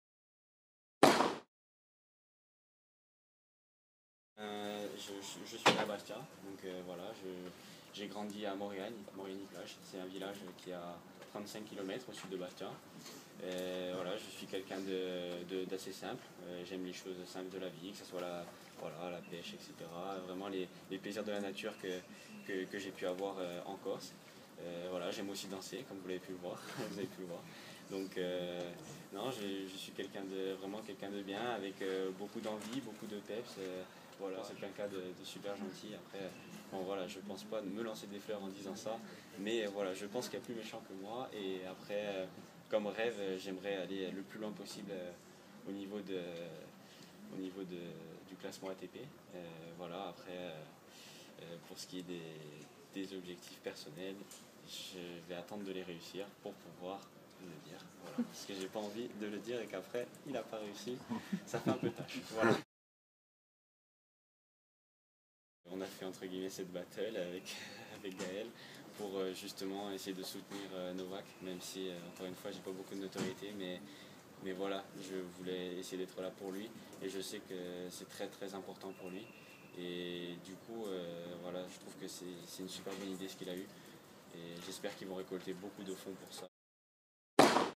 Découvrez Laurent Lokoli
Rencontre avec Laurent Lokoli, qualifié pour le grand tableau à Roland Garros !